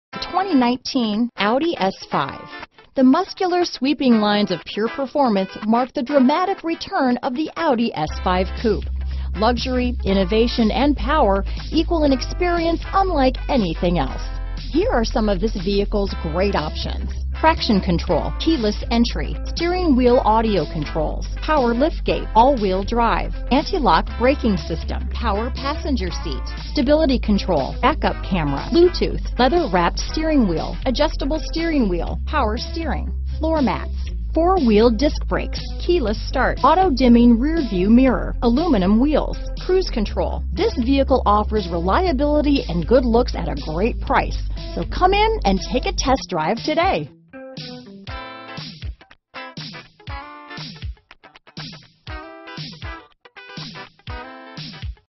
2019 Audi S5 Sportback Oxnard, Ventura, Camarillo, Thousand Oaks, Santa Barbara, CA AX04207
Navarra Blue Metallic New 2019 Audi S5 Sportback Available in Oxnard, California at DCH Audi Oxnard. Servicing the Ventura, Camarillo, Thousand Oaks, Santa Barbara, CA.
2019 Audi S5 Sportback Prestige - Stock#: AX04207 - VIN#: WAUC4CF52KA000579